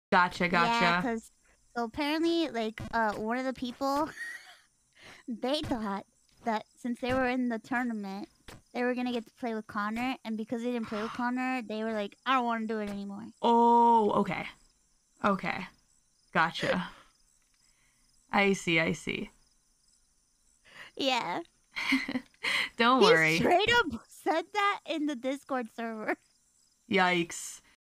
Creator was angry they weren't playing with Connor | Ironmouse Demondice
a creator was angry they weren't playing with Connor | Ironmouse Demondice